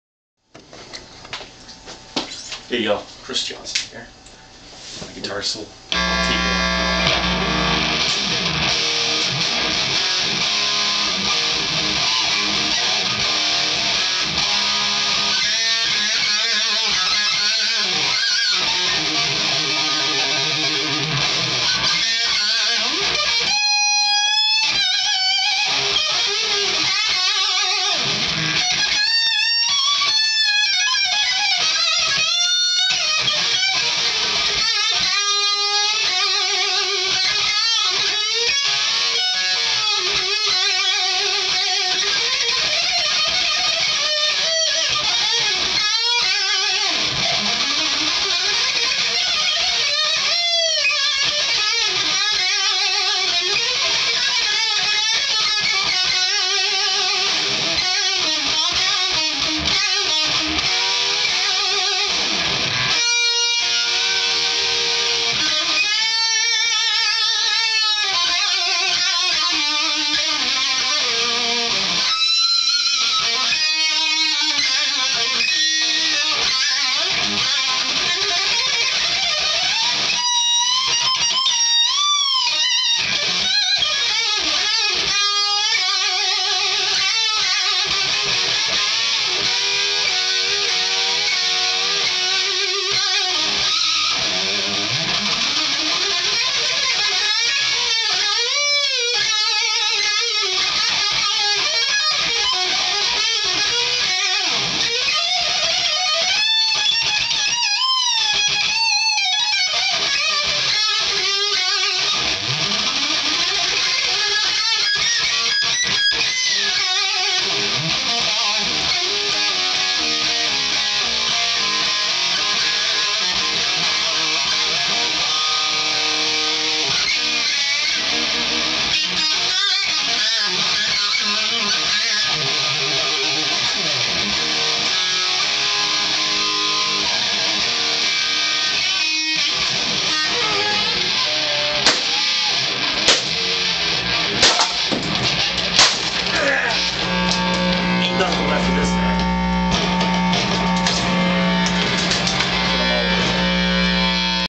Chris Johnson Smashing Solo
Bluesy Metal solo, with lots of emotion and raw power.  Very intense